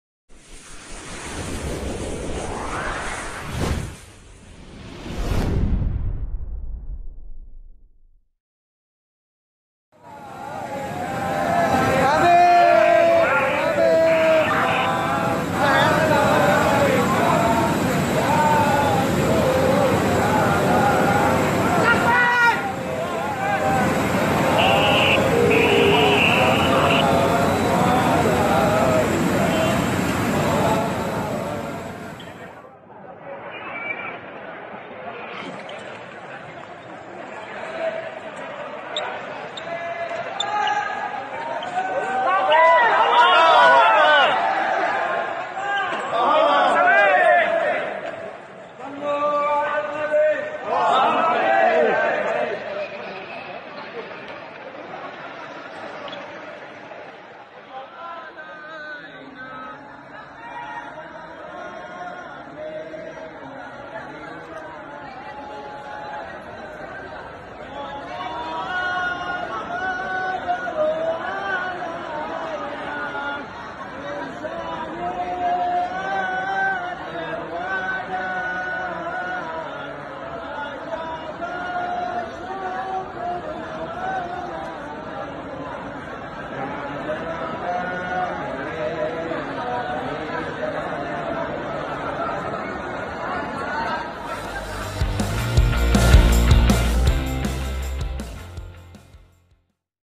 Para petugas Bandara menyambut kedatangan IB Habib Rizieq syihab dengan lantunan sholawat
terlihat di video banyak petugas bandara yang berdiri dan melantunkan sholawat untuk menyambut kedatangan Imam Besar Habib Rizieq syihab.